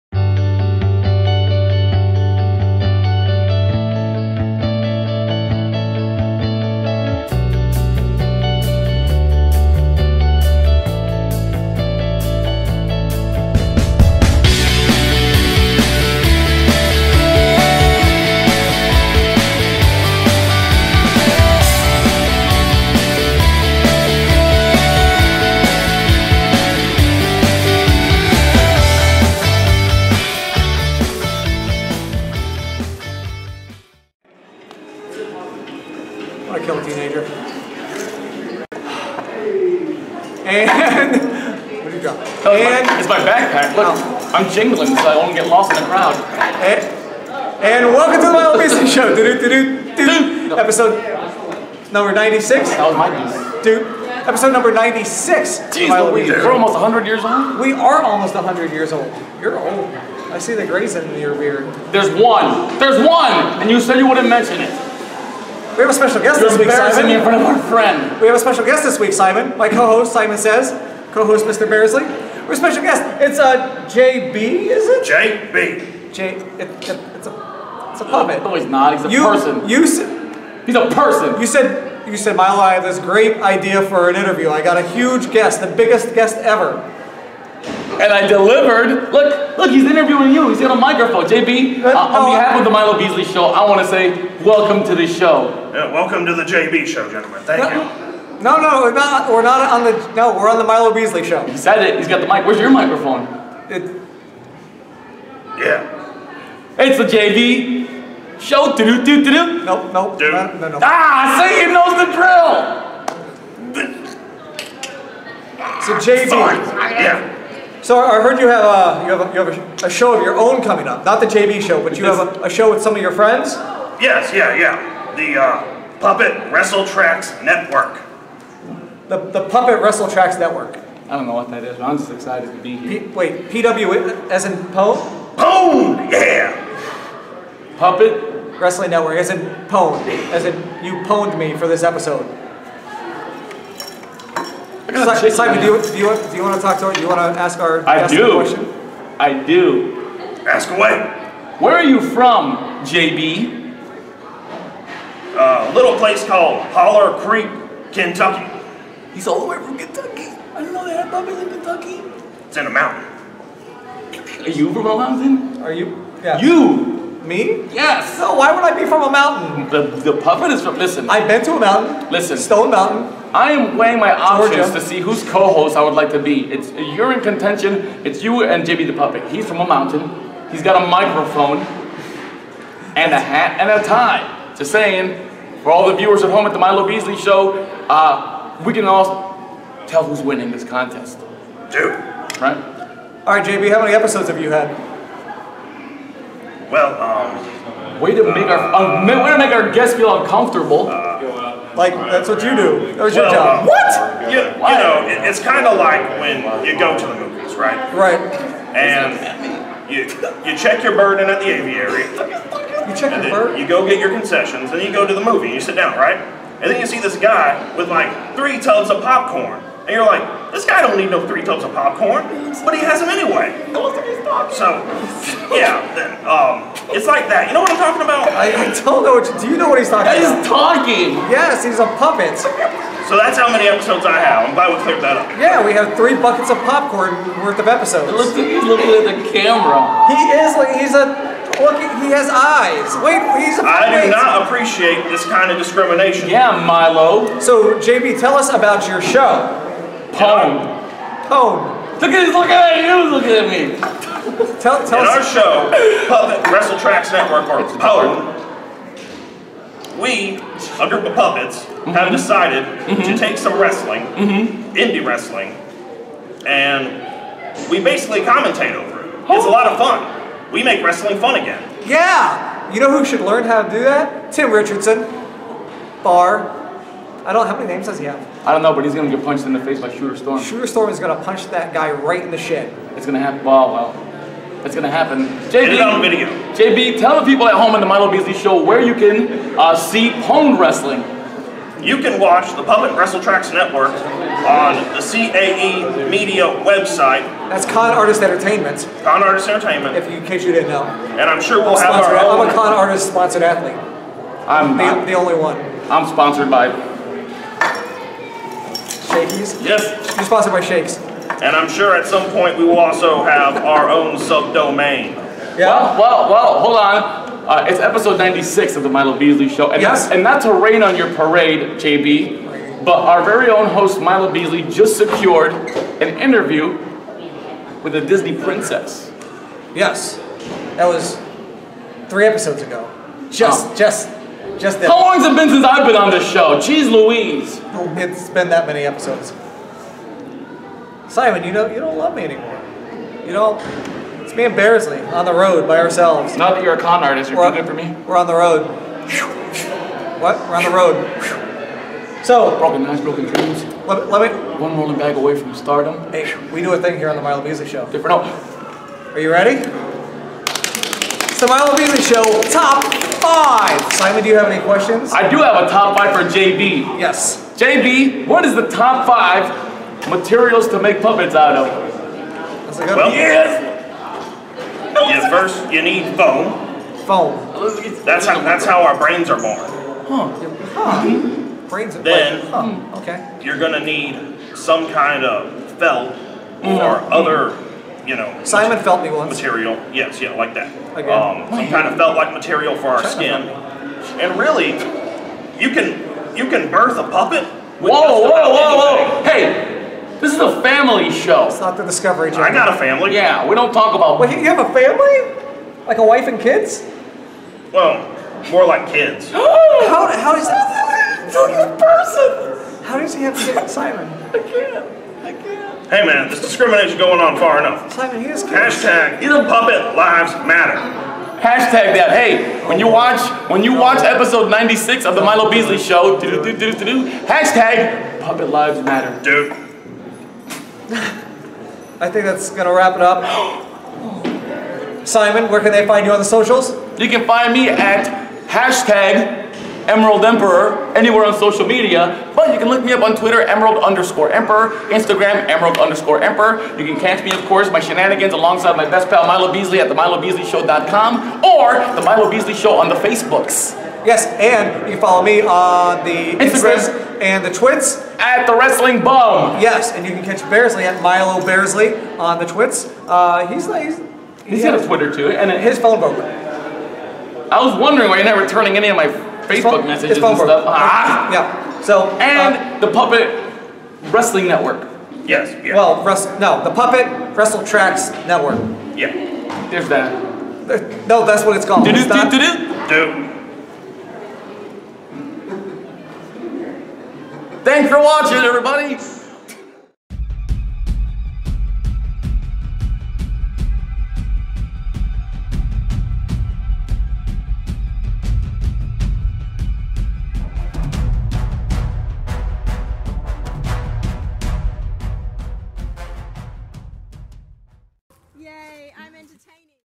The Milo Beasley Show episode 96 featuring JB from the Puppet Wrestletrax Network
Welcome to episode number 96 of The Milo Beasley Show! This week, Simon Sez joins us in the co-host seat as we welcome one of the stars of the Puppet Wrestletrax Network, JB!

You can also check out the PWN on YouTube and facebook.

Don't forget to give us a like on Facebook as well as follow us on the Twits and Instas @thewrestlingbum.

And remember... TELL YOUR FRIENDS!